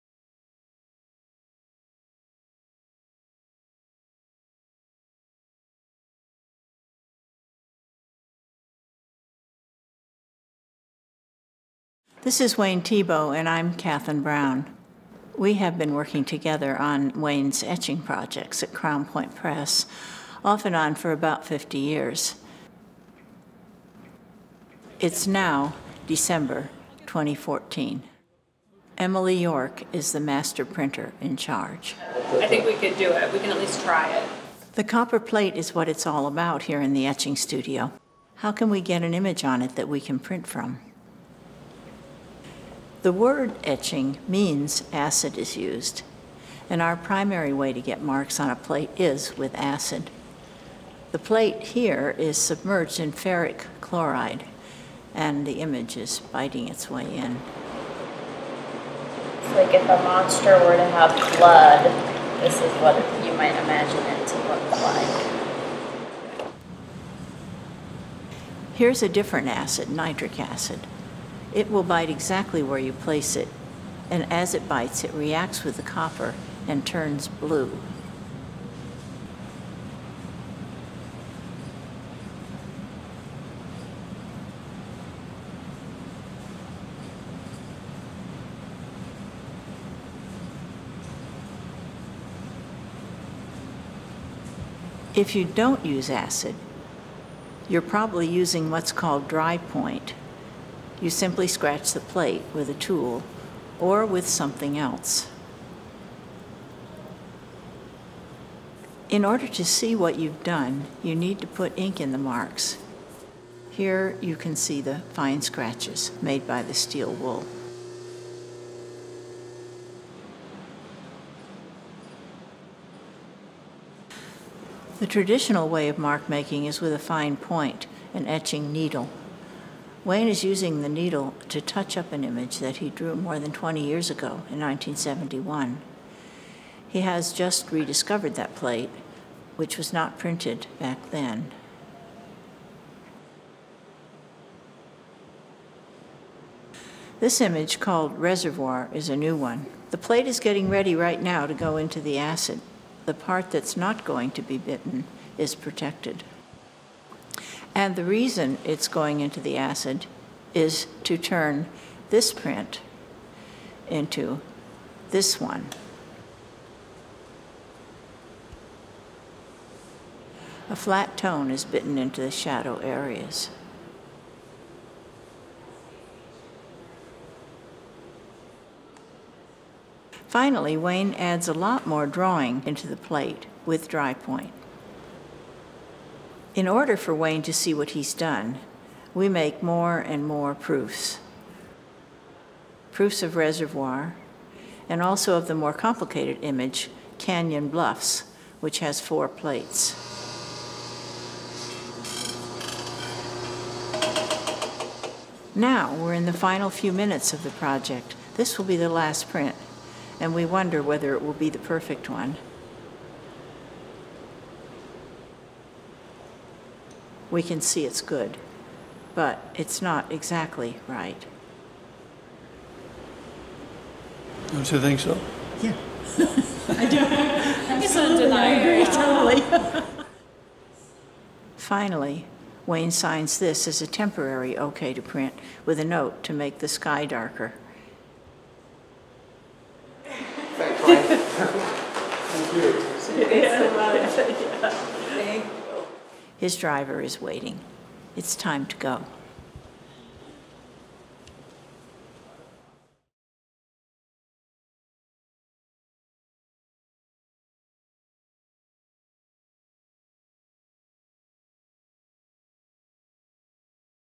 Wayne Thiebaud 2014 (4 minutes)
This video shows Wayne Thiebaud in the Crown Point Press etching studio.